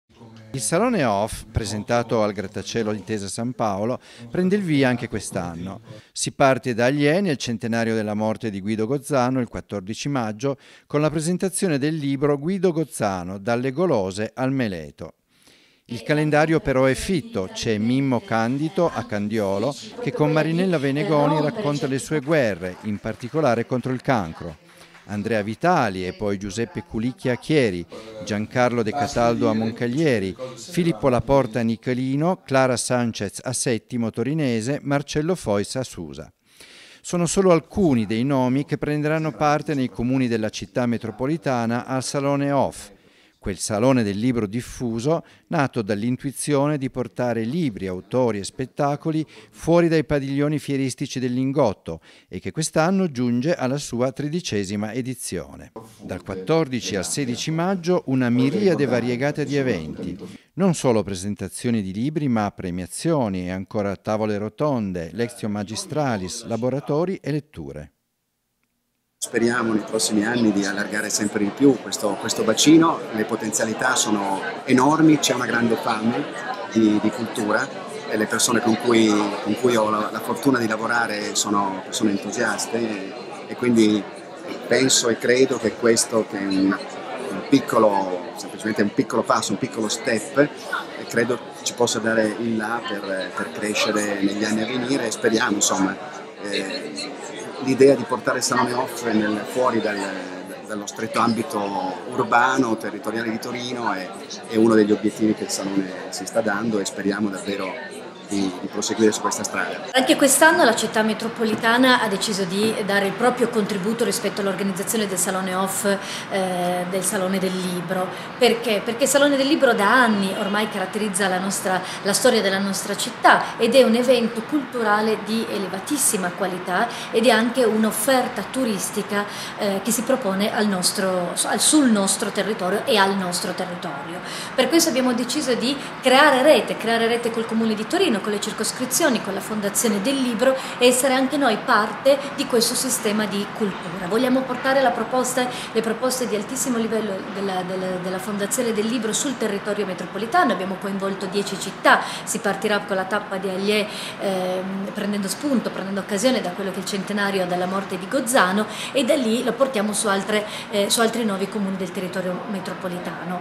Presentato il Salone Off
Presentato,  il ricco programma del Salone Off legato al Salone del libro